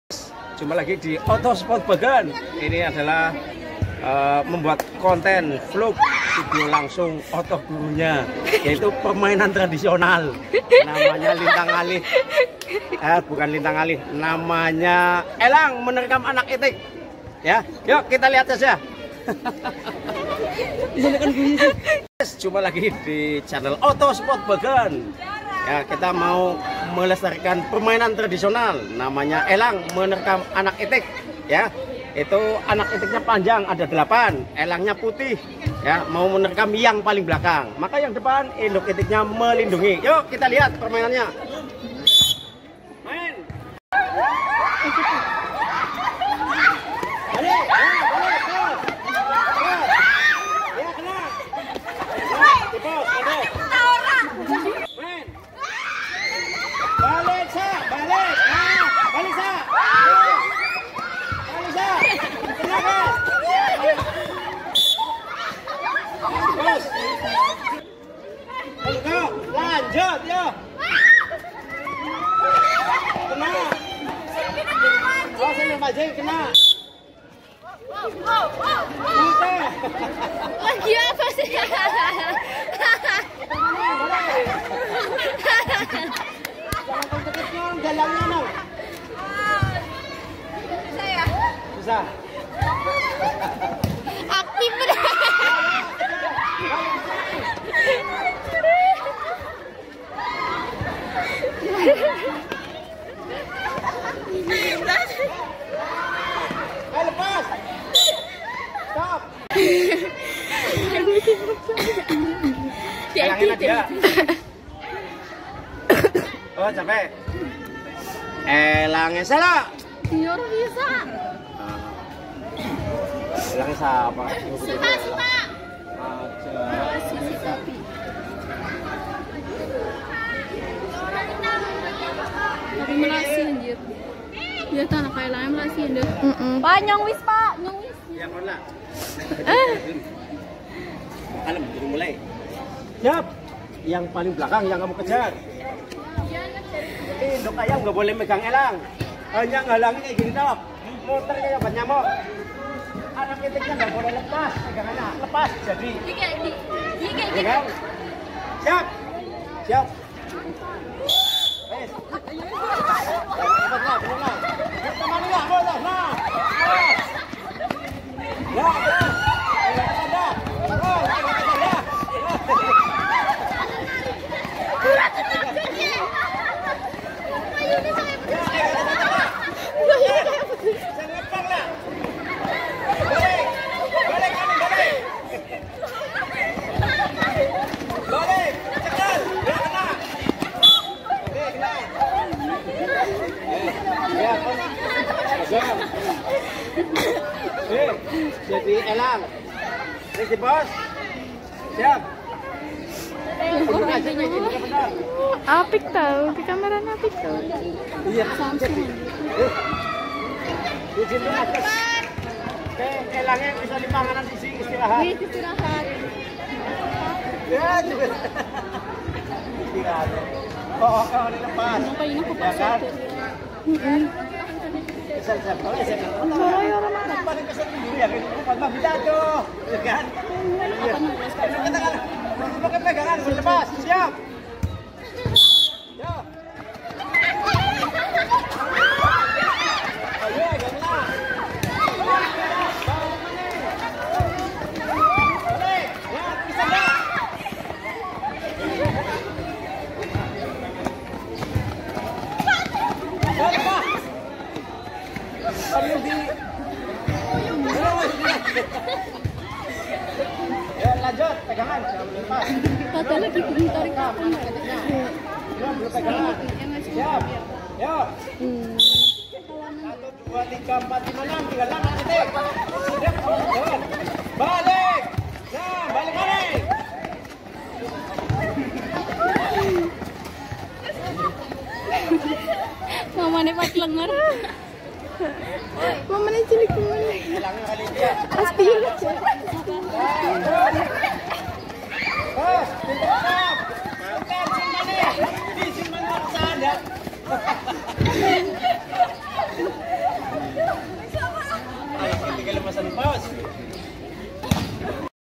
Elang menerkam itik xii.ipa3 ~ variasi pemanasan
permainan tradisional elang menerkam anak itik yaitu permainan anak anak indonesia peranindo